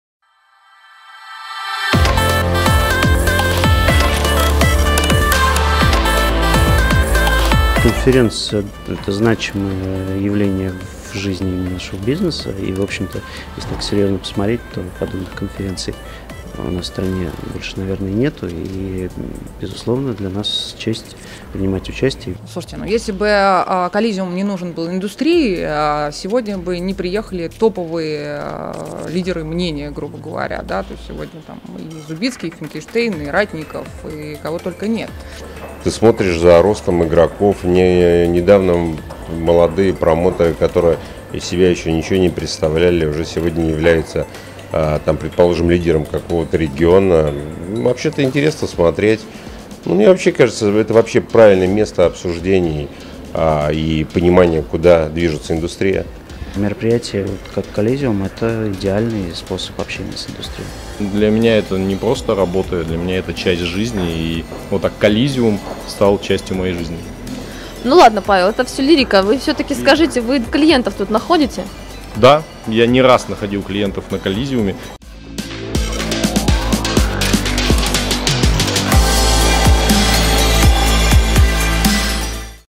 специалисты о Colisium Spb 2017
Что говорят специалисты индустрии о Colisium SPb 2017